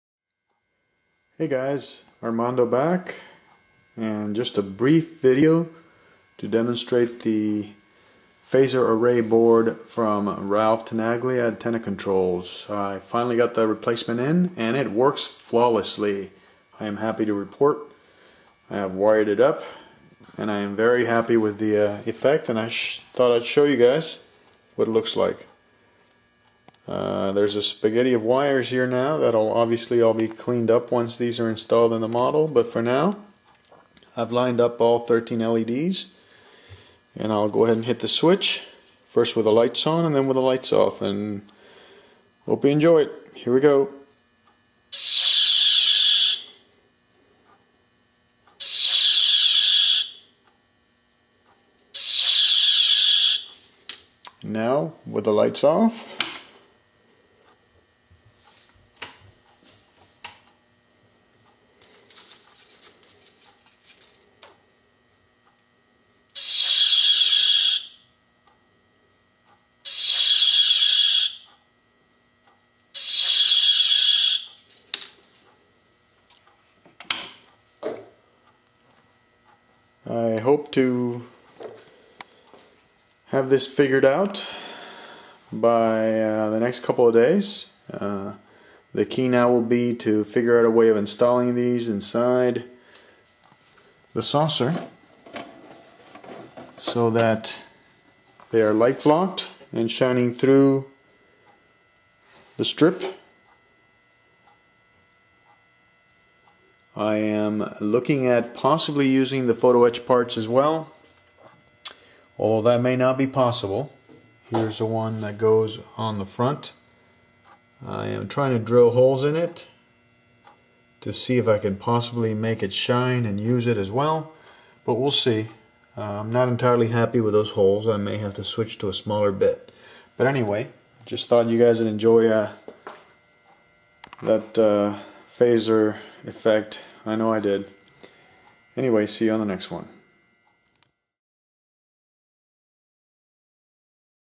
Enterprise D part 8
Testing the phaser array board from Tenacontrols